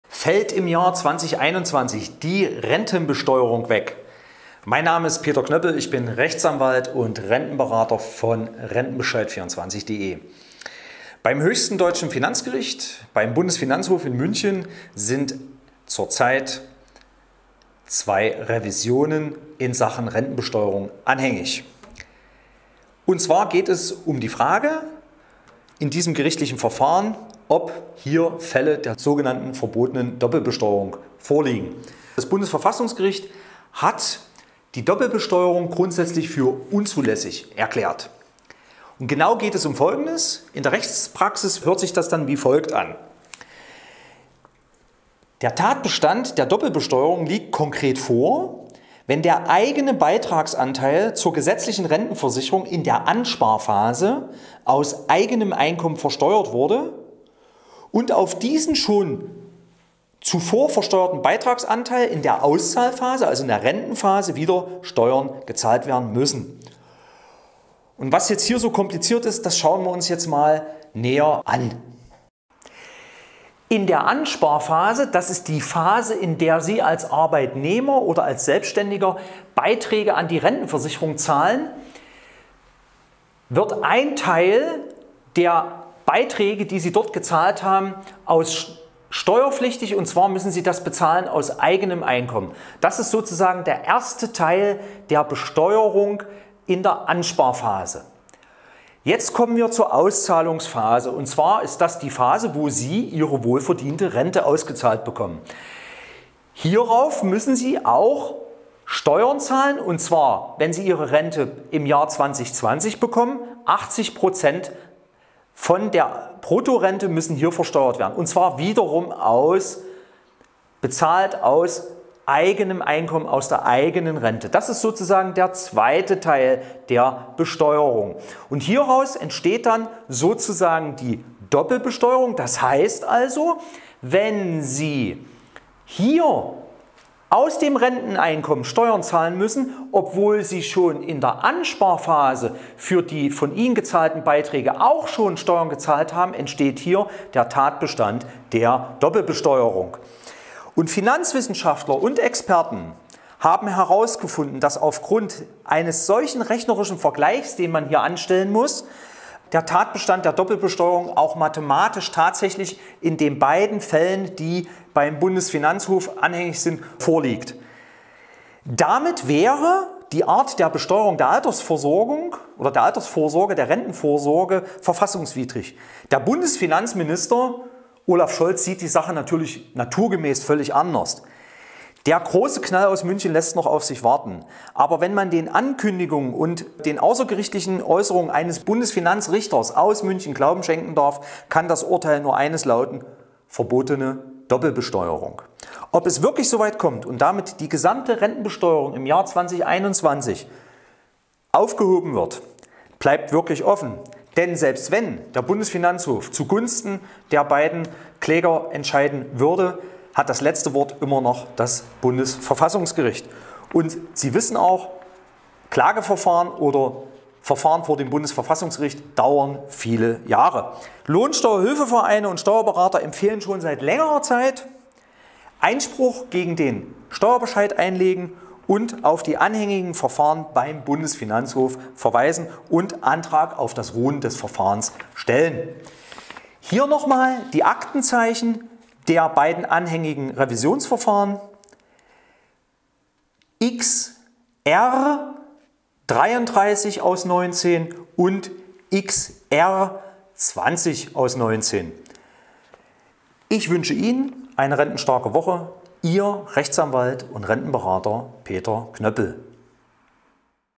Rente und Steuern - Fällt die Rentenbesteuerung ab 2021 weg
Ist womöglich ein Ende der Doppelbesteuerung in Sicht? Die Doppelbesteuerung ist verboten, doch bei den Rentenbeiträgen und schließlich bei der Auszahlung der Rente werden Steuern fällig. Nun befasst sich der Bundesgerichtshof mit dem Thema Rente und der Doppelbesteuerung. Daher gibt unser Rentenberater Knöppel von rentenbescheid24.de Tipps, was Sie unbedingt tun sollten.